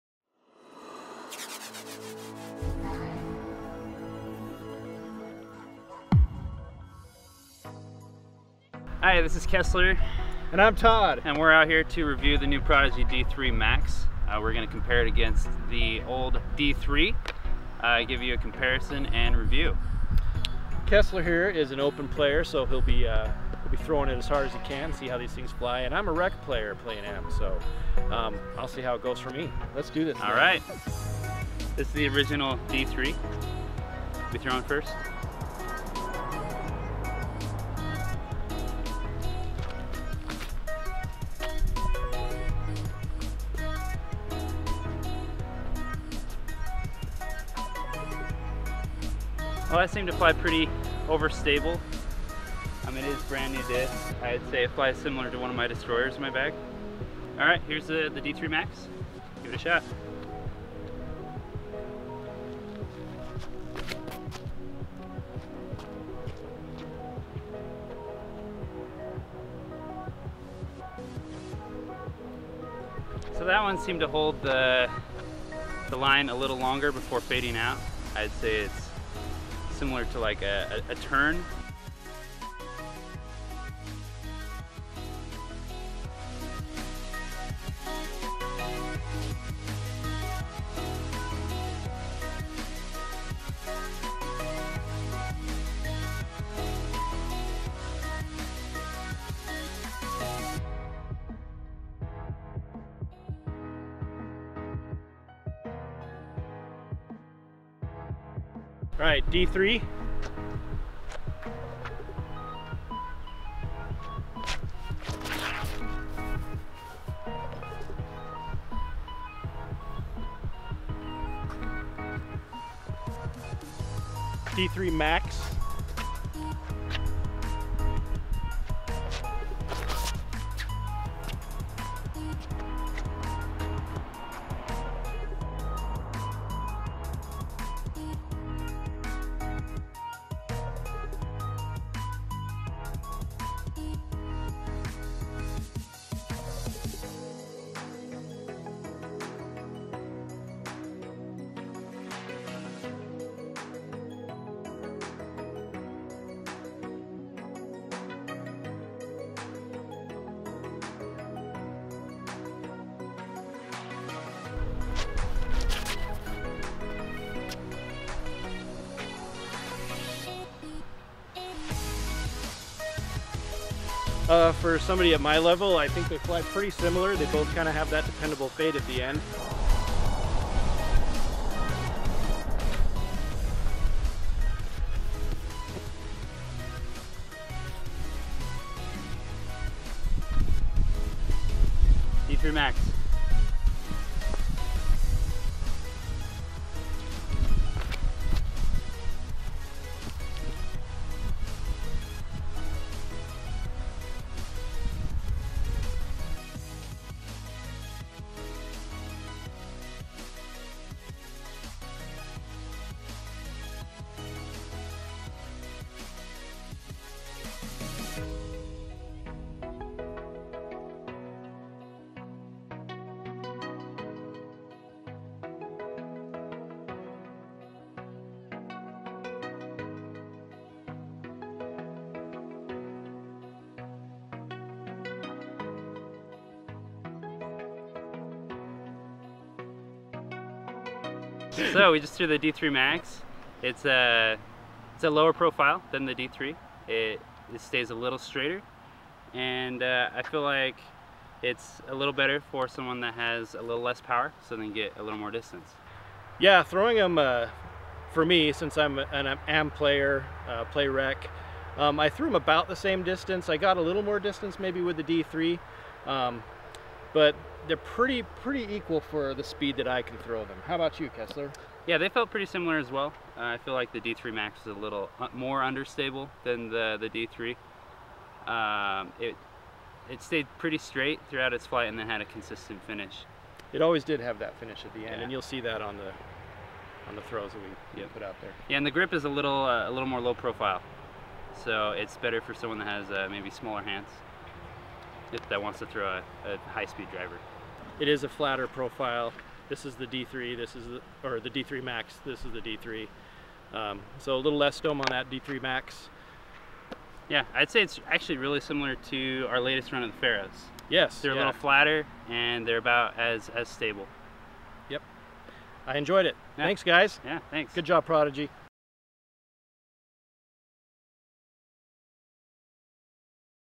Prodigy D3 Max Review & Comparison
Todd and Kesler try out the brand new Prodigy D3 Max (available They throw it in comparison with the original D3 mold.

Tell us what you think of the D3 Max!